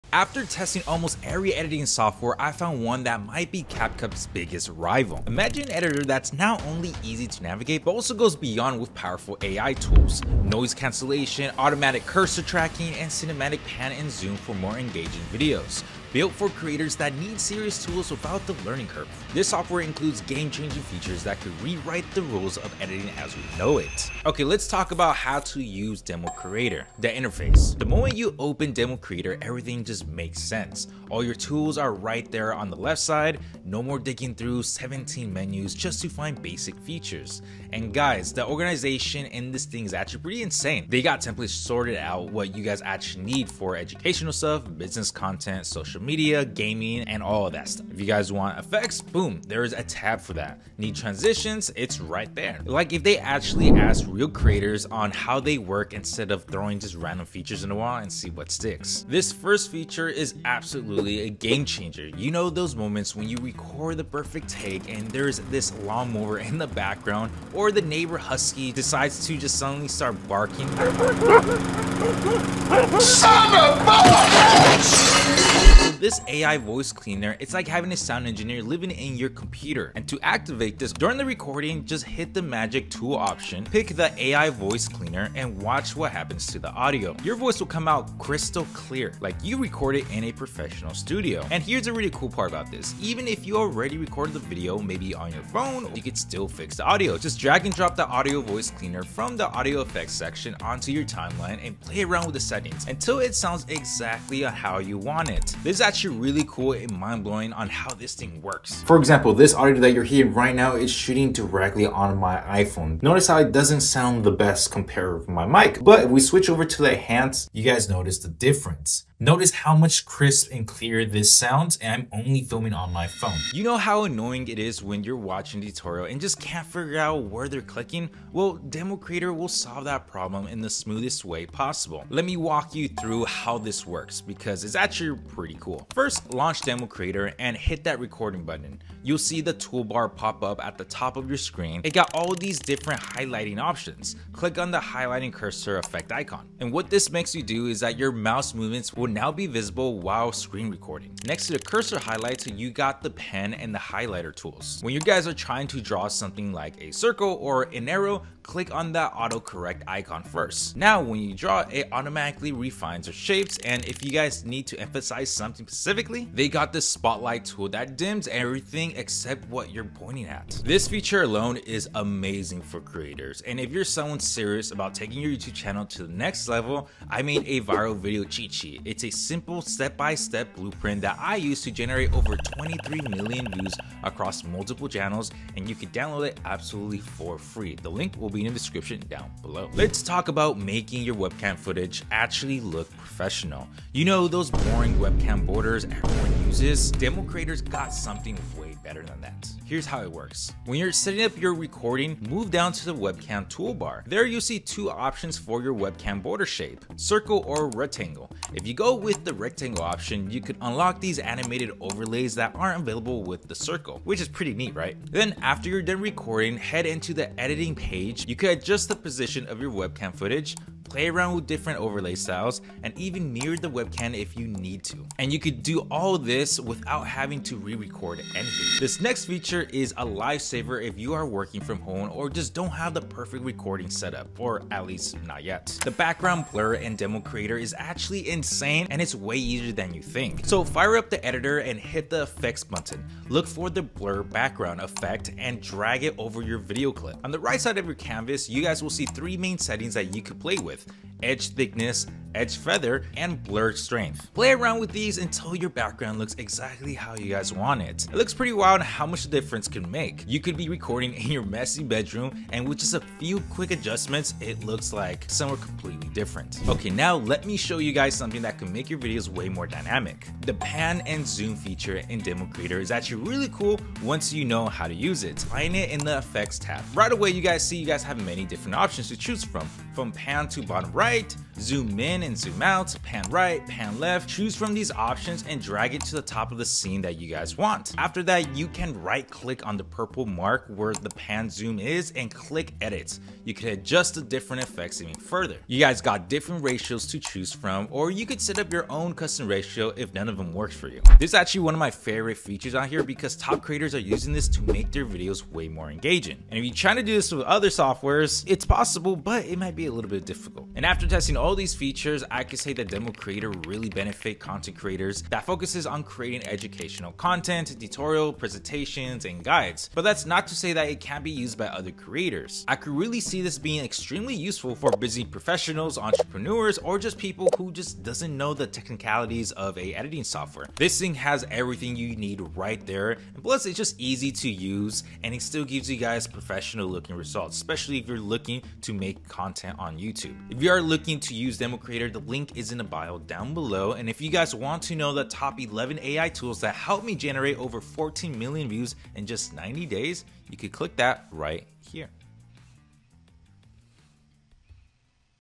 I Tested All the Best AI Editing Software and Found the Ultimate Winner!
Chapters
0:27 Interface
1:17 AI Voice Cleaner
2:13 Cursor Highlight
3:19 Overlay Effects
3:58 Background Blur
4:32 Pan & Zoom

After testing nearly every editing tool out there, I’ve found something that could be CapCut’s biggest competition: DemoCreator. This software is designed for creators who want powerful editing without the learning curve. It includes innovative features like AI-powered noise cancellation, automatic cursor tracking, and cinematic pan-and-zoom, making videos more dynamic and engaging.

While CapCut works well for quick TikToks or Reels, DemoCreator goes further for serious screen recording and instructional content. Its user-friendly interface is organized with templates for educational, business, and gaming videos, giving creators exactly what they need, right where they need it.

One standout feature is the AI Voice Cleaner. It clears background noise—perfect if your recording gets interrupted by sounds like lawn mowers or pets. DemoCreator also makes tutorial creation effortless with cursor highlighting, spotlight effects, and auto-correct tools, keeping your audience focused and informed.

Webcam overlay options add a professional touch, and with easy background blurring, you can achieve a polished look even from a messy room. Plus, the Smart Pan & Zoom tool lets you add movement and depth to your content without a hassle.

DemoCreator is ideal for educators, tutorial creators, and professionals who want high-quality, instructional content with minimal setup. It's intuitive, accessible, and equipped with powerful features that make your content shine.